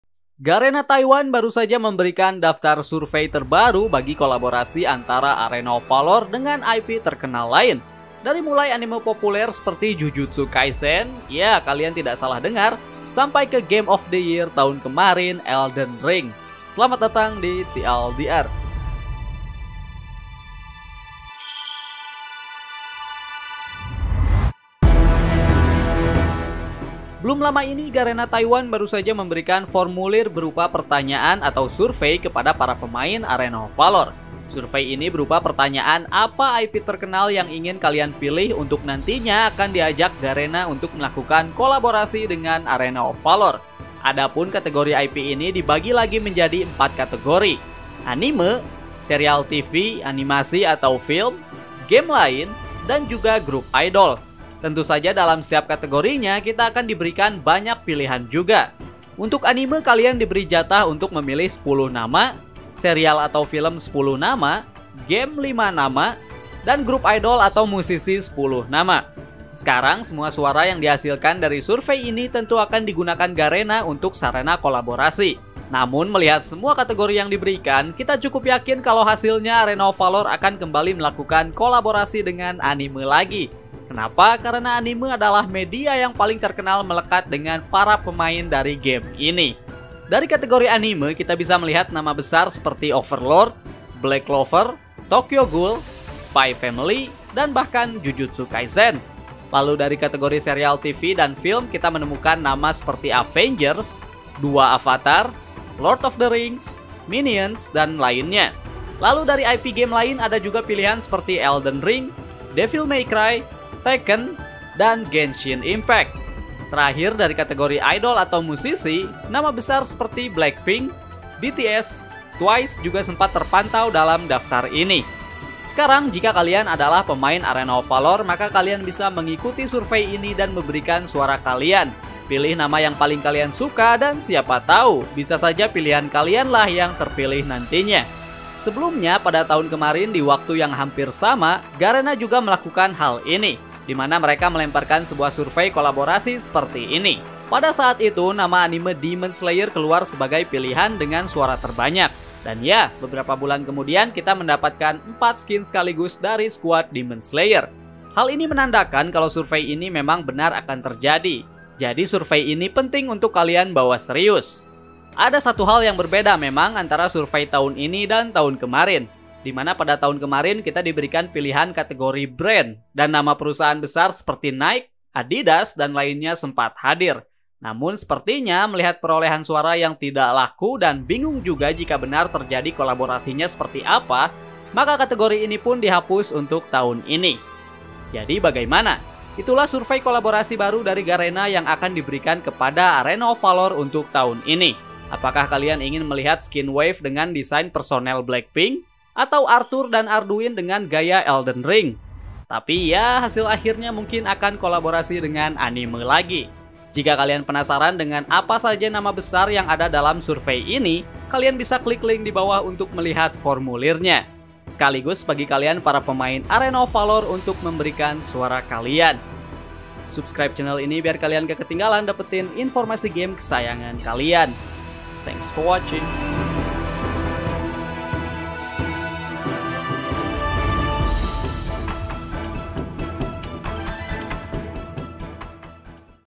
Garena Kembali Berikan Survey Kolaborasi Besar Bagi Arena of Valor! - TL;DR
Follow Us for More Gaming & Esports News,
Pick Your Game and Enjoy Our Content.